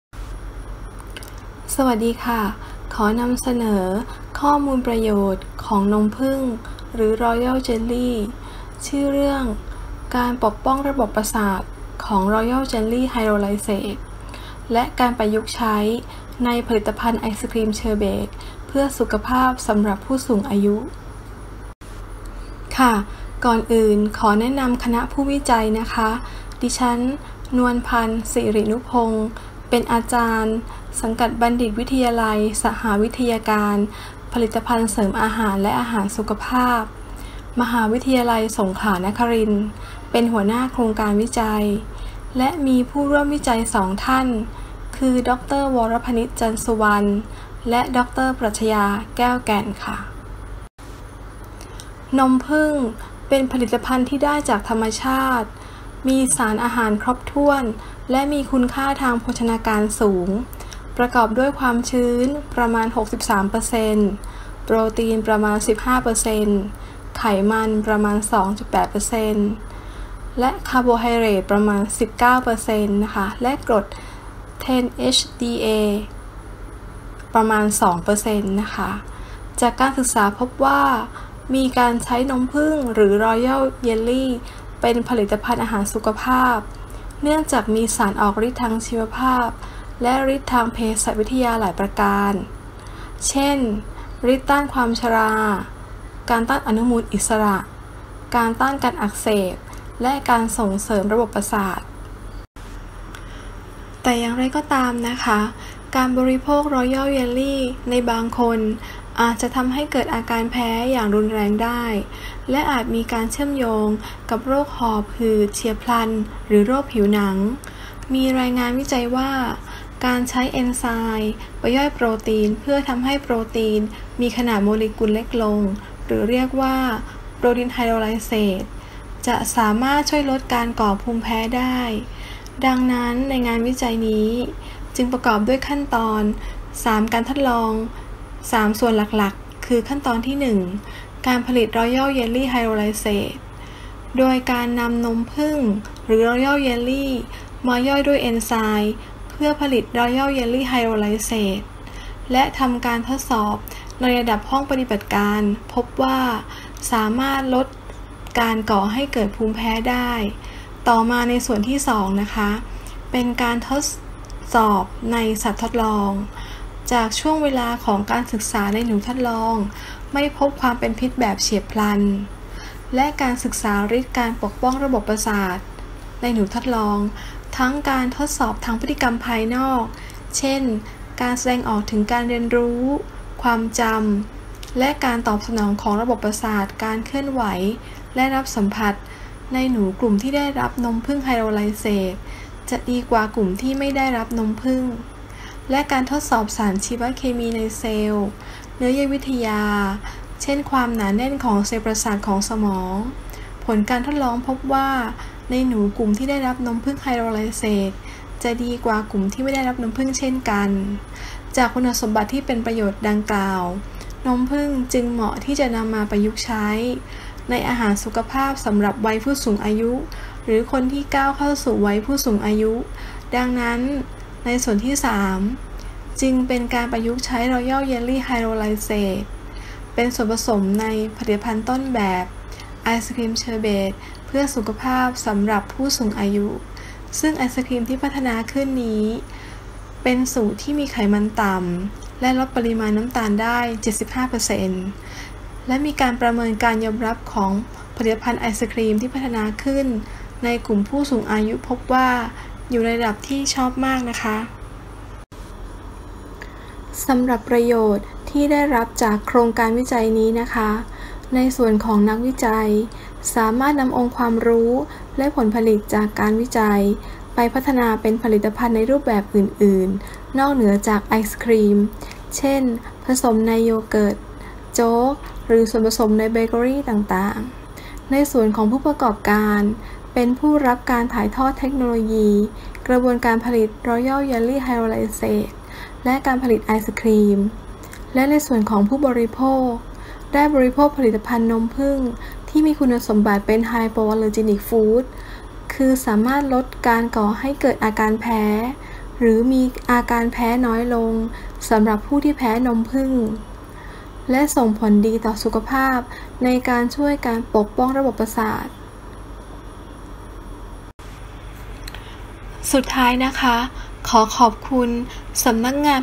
ผลิตภัณฑ์ไอศครีมเสริมด้วยรอยัลเยลลี่ไฮโดรไลเสท (นมผึ้ง) | สวก.
ผลิตภัณฑ์ไอศครีมเสริมด้วยรอยัลเยลลี่ไฮโดรไลเสท

         การปกป้องระบบประสาทของรอยัลเยลลี่ไฮโดรไลเสทและการประยุกต์ใช้ในผลิตภัณฑ์ไอศครีมเชอร์เบทเพื่อสุขภาพสำหรับผู้สูงอายุ

        นมผึ้งเป็นผลิตภัณฑ์ที่ได้จากธรรมชาติ พัฒนาการสูง มีสารอาหารครบถ้วน และมีคุณค่าทางโภชนาการสูง ประกอบด้วยความชื้นประมาณ 63 % โปรตีนประมาณ 15 % ไขมันประมาณ 2.8%  คาร์โบไฮเดรตประมาณ 19% และกรด 10-HDA 2 %

        จากการศึกษาพบว่ามีการใช้นมผึ้งหรือรอยัลเยลลี่ เป็นผลิตภัณฑ์อาหารสุขภาพ เนื่องจากมีสารออกฤทธิ์ทางชีวภาพ ทางเภสัชวิทยาหลายประการ เช่นฤทธิ์ต้านความชรา การต้านอนุมูลอิสระ การต้านการอักเสบ และการส่งเสริมระบบประสาท

         ในบางท่านอาจจะเกิดอาการแพ้อย่างรุนแรงได้ และอาจมีการเชื่อมโยงกับโรคหอบหืดเฉียบพลัน หรือโรคติดผิวหนัง มีงานวิจัยว่าการใช้เอนไซม์ไปย่อยโปรตีน เพื่อให้โปรตีนมีขนาดโมเลกุลเล็กลง หรือเรียกว่า protein hydrolysis จะสามารถช่วยลดการก่อภูมิแพ้ได้ ดังนั้นในการวิจัยนี้จึงประกอบด้วย 3 การทดลอง คือขั้นตอนที่ 1  การผลิตรอยัลเยลลี่  โดยการนำนมผึ้งหรือย่อยที่มาย่อยด้วยเอนไซม์เพื่อผลิตรอยัลเยลลี่ hydrolysis ลดการก่อให้เกิดภูมิแพ้ได้  ส่วนที่ 2 เป็นการทดสอบในสัตว์ทดลอง ไม่พบความเป็นพิษแบบเฉียบพลัน และในส่วนที่ 3 เป็นการนำนมผึ้งมาประยุกต์โดยผลิตเป็นผลิตภัณฑ์ไอศครีมเสริมด้วยรอยัลเยลลี่ไฮโดรไลเสท ที่เหมาะสำหรับผู้สูงอายุและผู้ที่กำลังจะเข้าสู่วัยผู้สูงอายุ เพราะมีไขมันต่ำ ลดน้ำตาลได้มากกว่า 75 %

        นักวิจัยสามารถนำองค์ความรู้และผลผลิตจากงานวิจัยไปพัฒนาเป็นผลิตภัณฑ์ในรูปแบบอื่นได้ ผู้ประกอบการเป็นผู้รับการถ่ายทอดเทคโนโลยีกระบวนการผลิต และการผลิตไอศครีม ส่วนผู้บริโภคได้บริโภคผลิตภัณฑ์นมผึ้งที่มีคุณภาพเป็นไฮโปรโทนิก food และส่งผลดีต่อสุขภาพในการช่วยปกป้องระบบประสาท